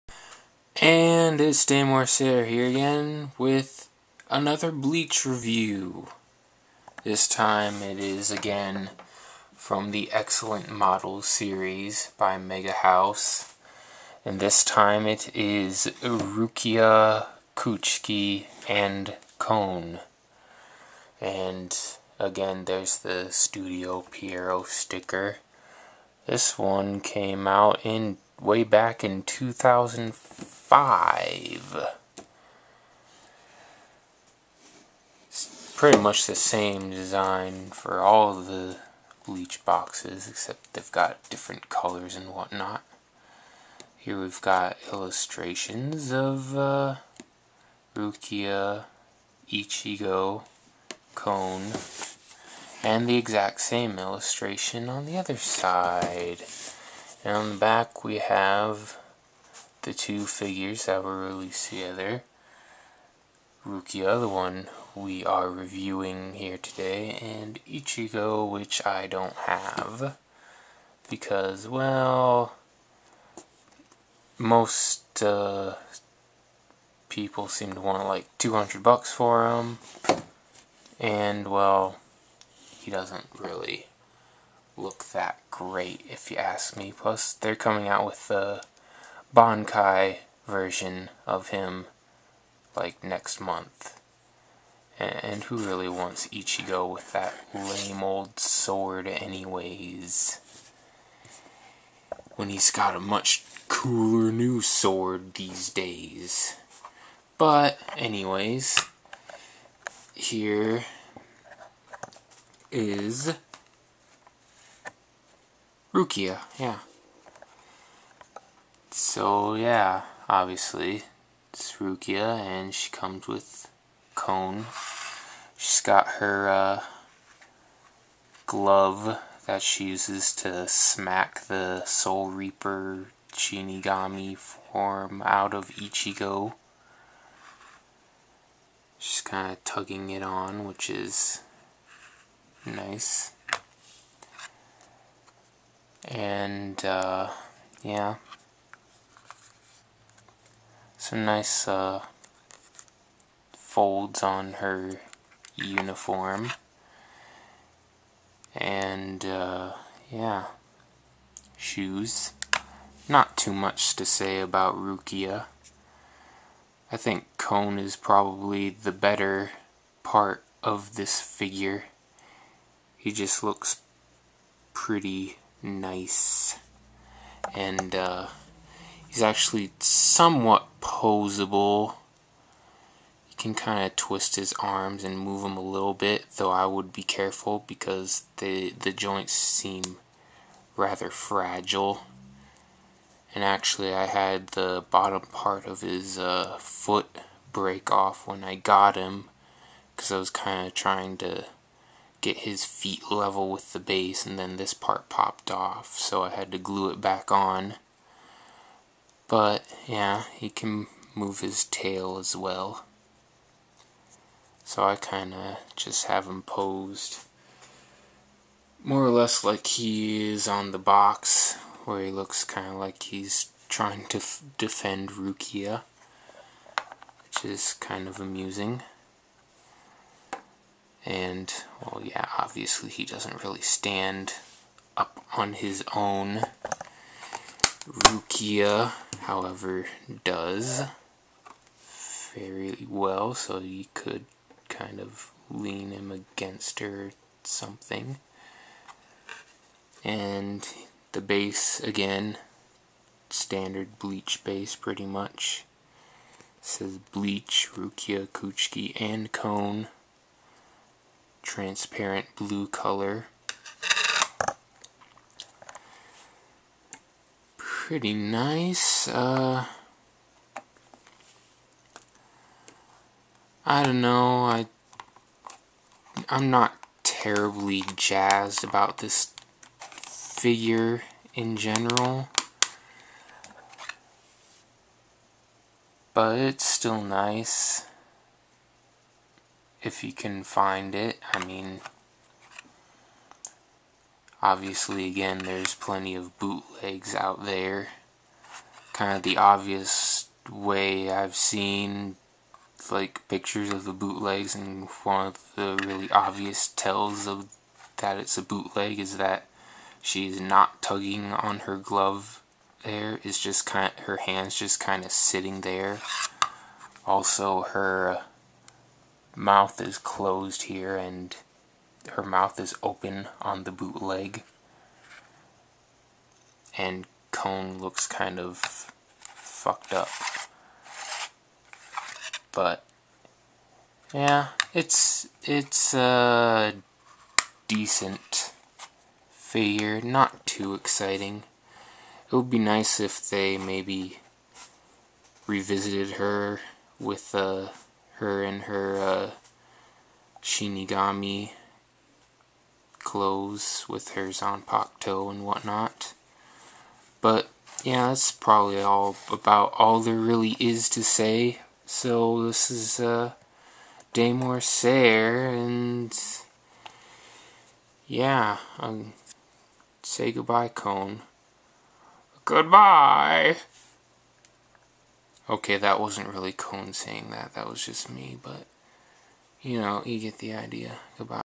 Excellent Model Rukia Kuchiki & Kon Review
More Bleach reviews?  Why, yes!  This time it's Rukia Kuchiki and that mischievous little stuffed animal, Kon.